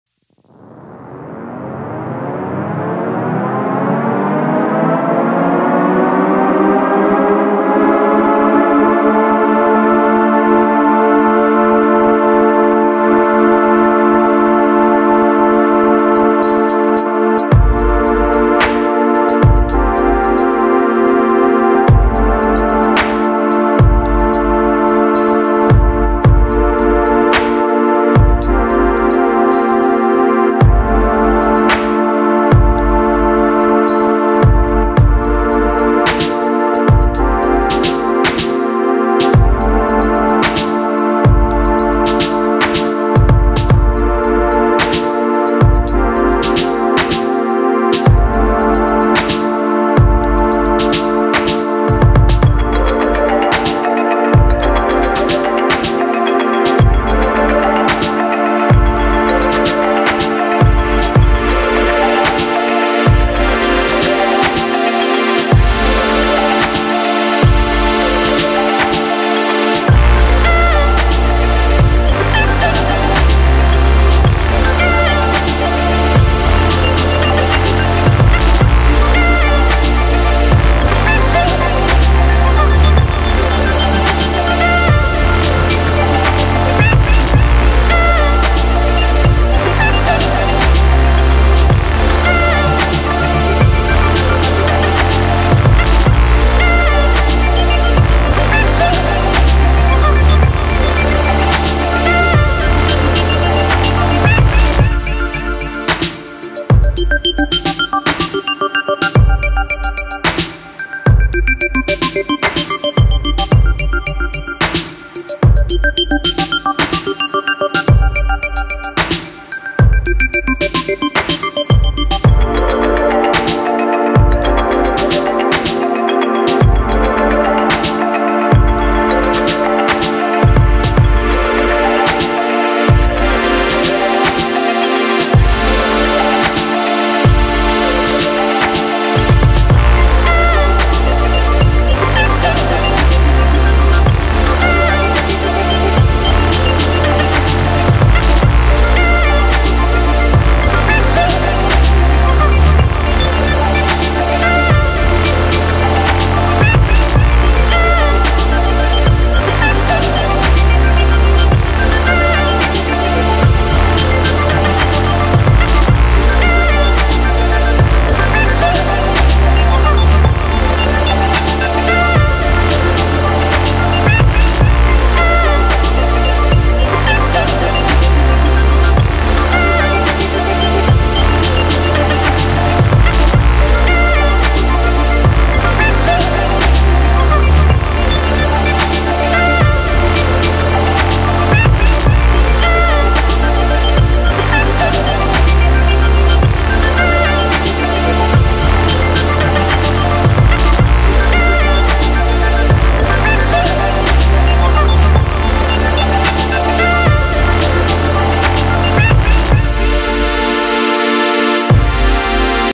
Hip Hop Music & Writing on a Skate Park - Phantom 3 Video
Phantom 3 Video inside a skate park: writing and hip hop music video.